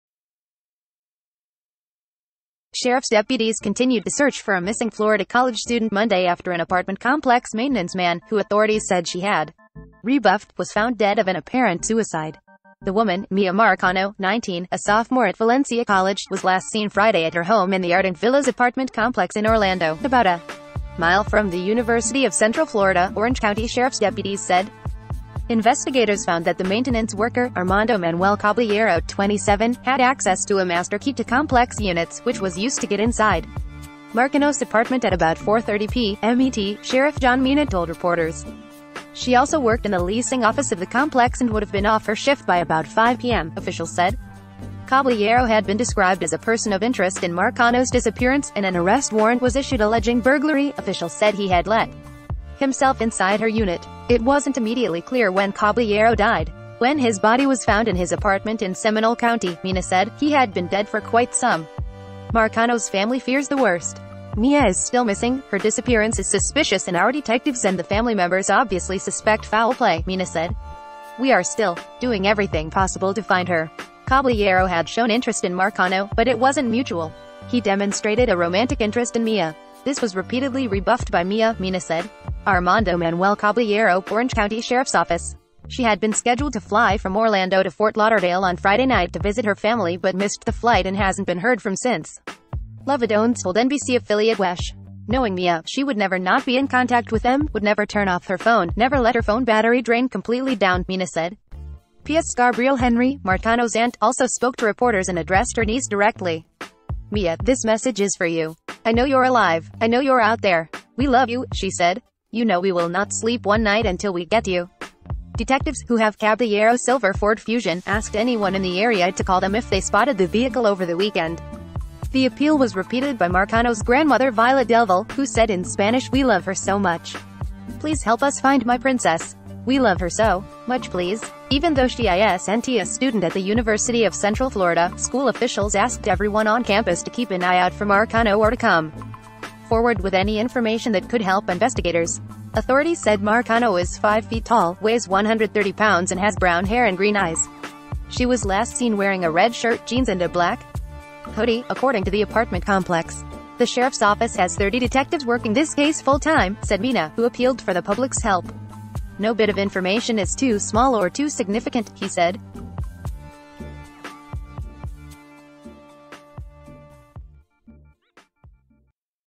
Person of interest found dead in search for Miya Marcano, missing Florida college student
Sheriff's deputies continued to search for a missing Florida college student Monday after an apartment complex maintenance man — who authorities said she had"rebuffed"— was found dead of an apparent suicide.The woman, Miya Marcano, 19, a sophomore at Valencia College, was last seen Friday at her home in the Arden Villas apartment complex in Orlando, about amilefrom the University of Central Florida, Orange County sheriff's deputies said.Investigators found that the maintenance worker, Armando M credit